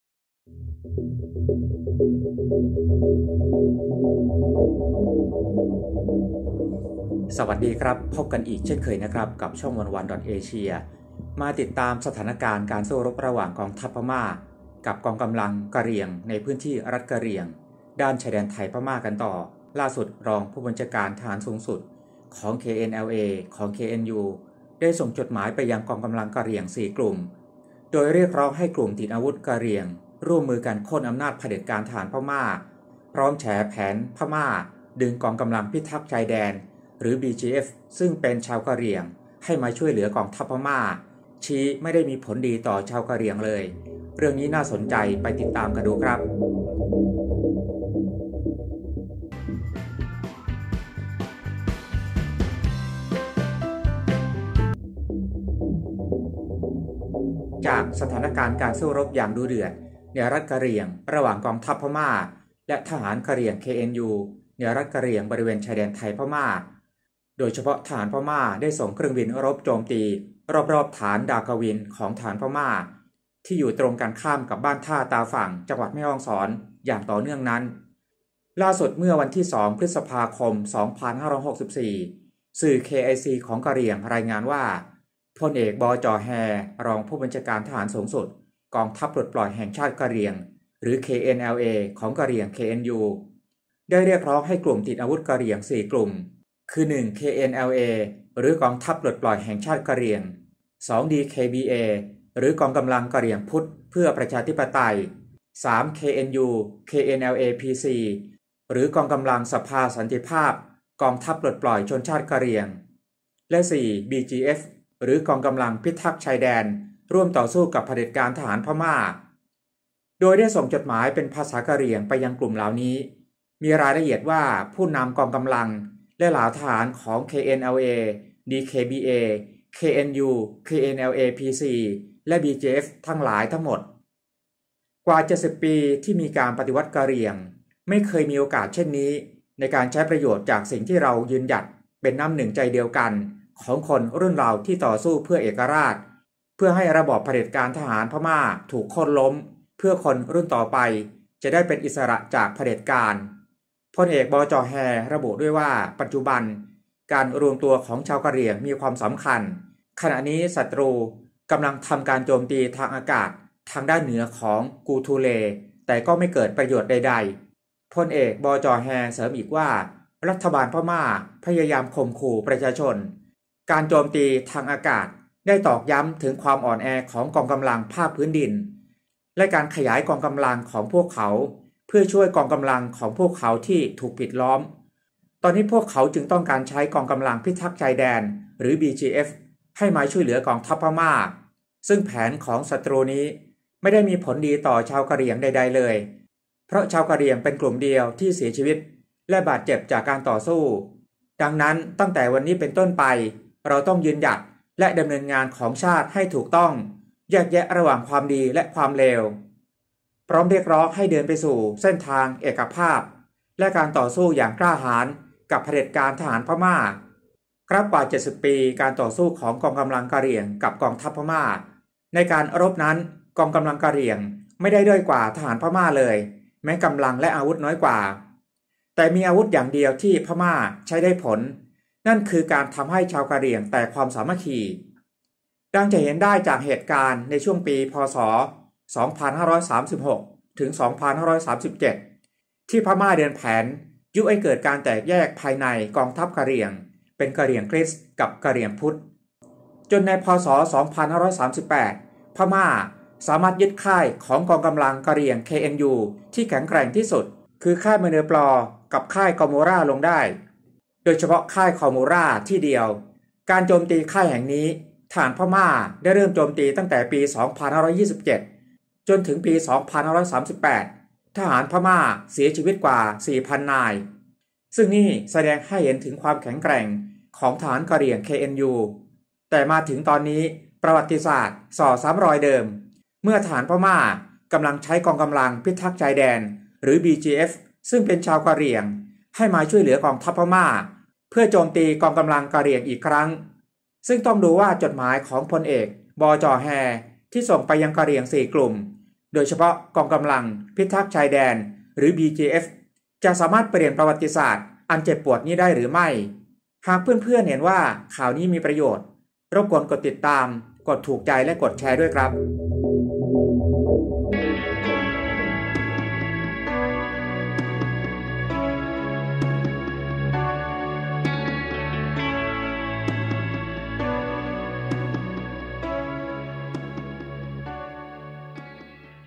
KNU ส่งจม. ปลุก "4 กองกำลังกะเหรี่ยง"ร่วมลุยโค่นเผด็จการทหารพม่า
รองผู้บัญชาการทหารสูงสุด KNLA ของ KNU ได้ส่งจดหมายไปยัง กองกำลังกะเหรี่ยง 4 กลุ่ม โดยเรียกร้องให้กลุ่มติดอาวุธกะเหรี่ยงร่วมมือกันโค่นอำนาจเผด็จการทหาร พร้อมแฉแผน พม่าดึงกองกำลังพิทักษ์ชายแดน (BGF ) ซึ่งเป็นชาวกะเหรี่ยง ให้มาช่วยเหลือกองทัพพม่า ชี้ไม่ได้มีผลดีต่อชาวกะเหรี่ยงเลย เรื่องนี้น่าสนใจไปติดตามกันครับ คลิกอ่านรายละเอียด